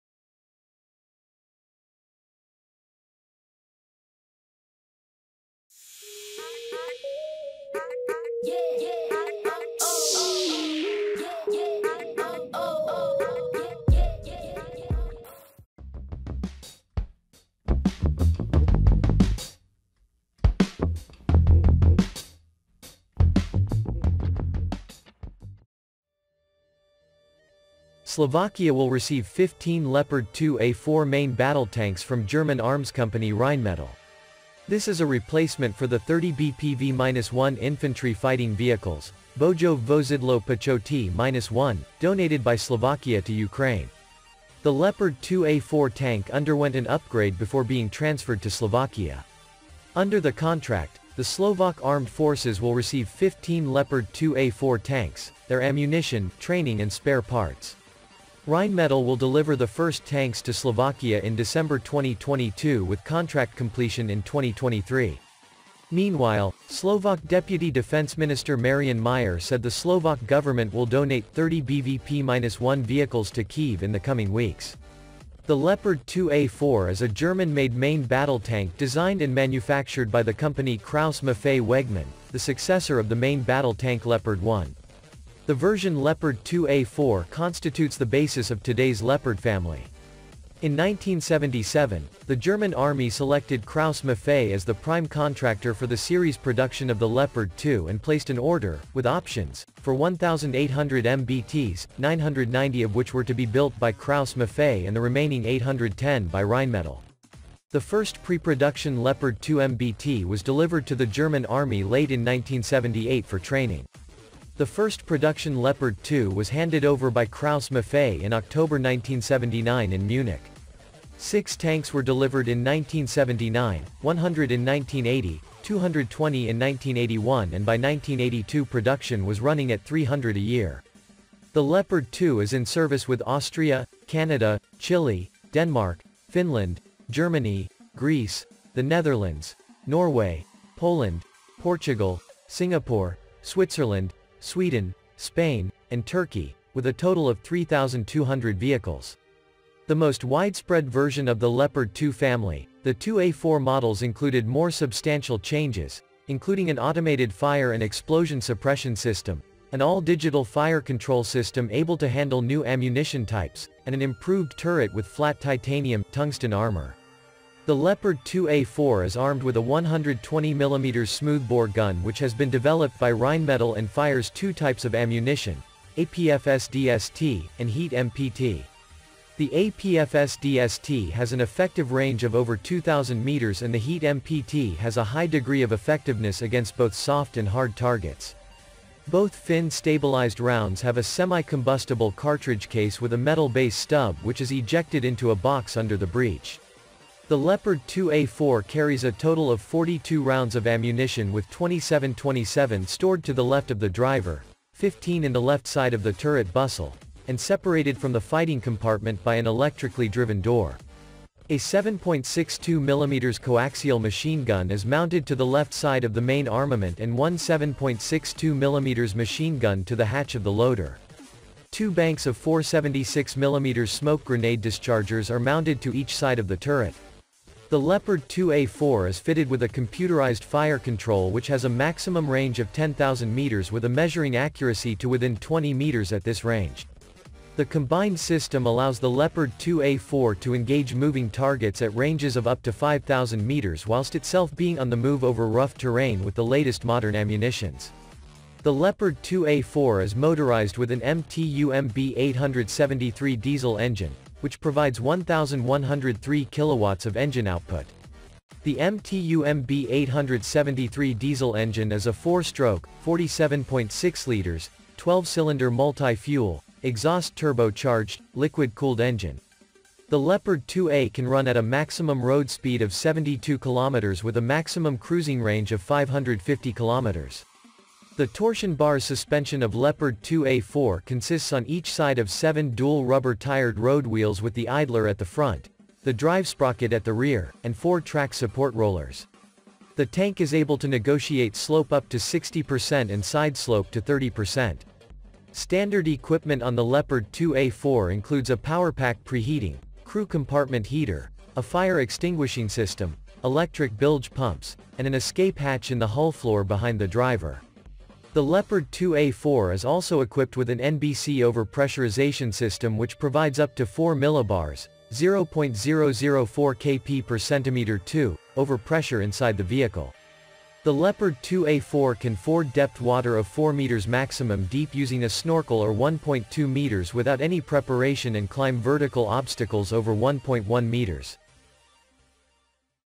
Slovakia to receive 15 Leopard 2A4s from German arms company Rheinmetall
Slovakia will receive 15 Leopard 2A4 main battle tanks from German arms company Rheinmetall.
This is a replacement for the 30 BPV-1 infantry fighting vehicles ( Bojove Vozidlo Pechoty -1) donated by Slovakia to Ukraine.
The Leopard 2A4 tank underwent an upgrade before being transferred to Slovakia.
Under the contract, the Slovak Armed Forces will receive 15 Leopard 2A4 tanks , their ammunition, training and spare parts.
Rheinmetall will deliver the first tanks to Slovakia in December 2022 with contract completion in 2023.
Meanwhile, Slovak Deputy Defense Minister Marian Majer said the Slovak government will donate 30 BVP-1 vehicles to Kyiv in the coming weeks.
The Leopard 2A4 is a German-made main battle tank designed and manufactured by the Company Krauss-Maffei Wegmann, the successor of the main battle tank Leopard 1.